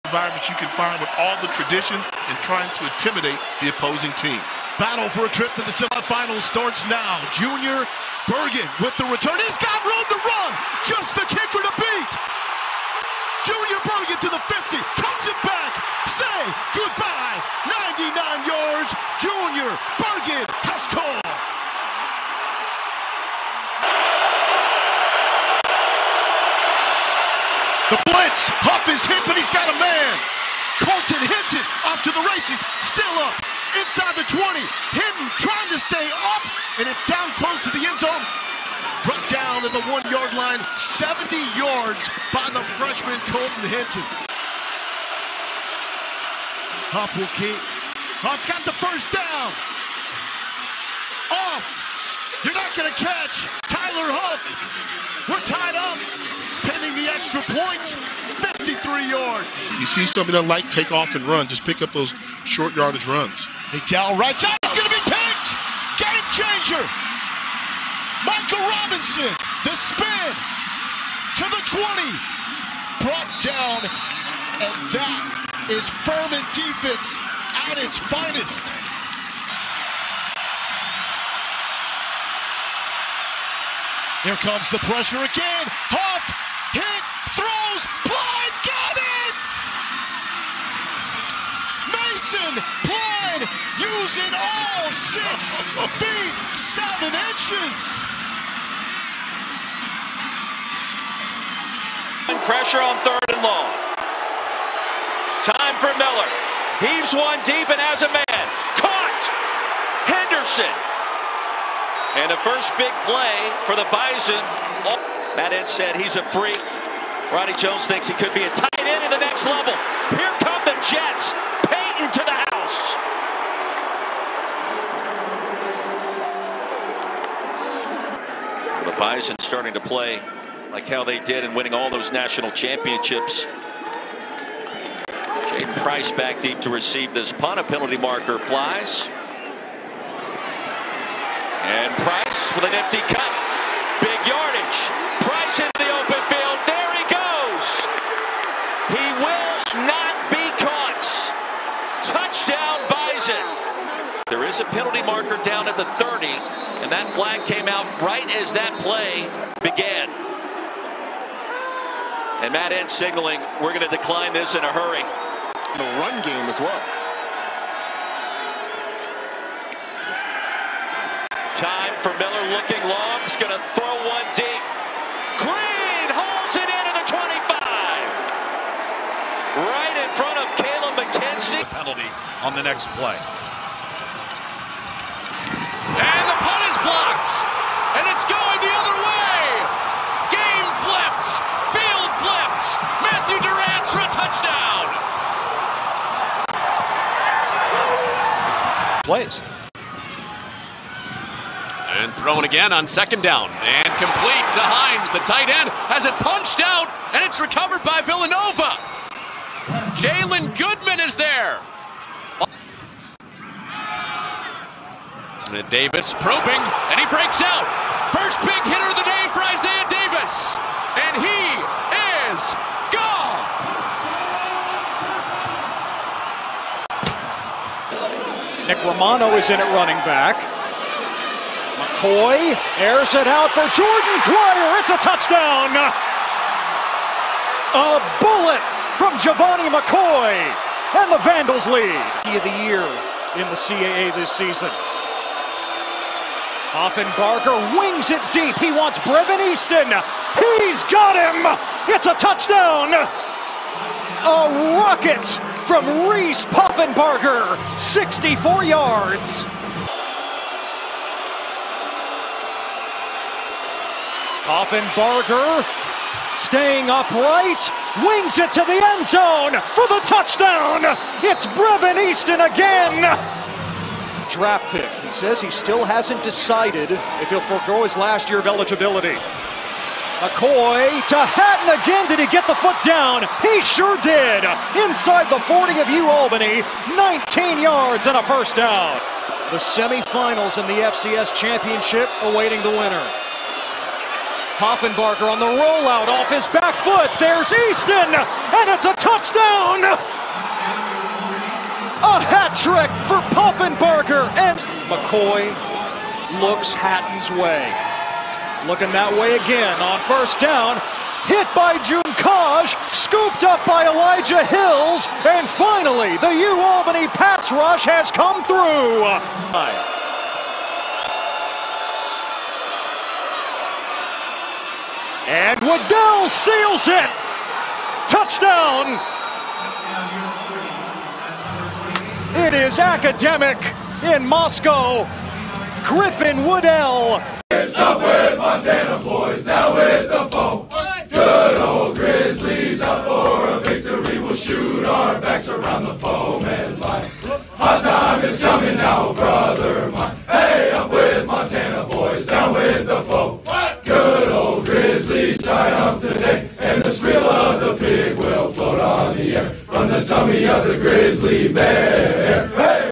Best Plays of FCS Playoffs Round 3
From electric kick returns to incredible throws, these are the best plays of FCS Playoffs round 3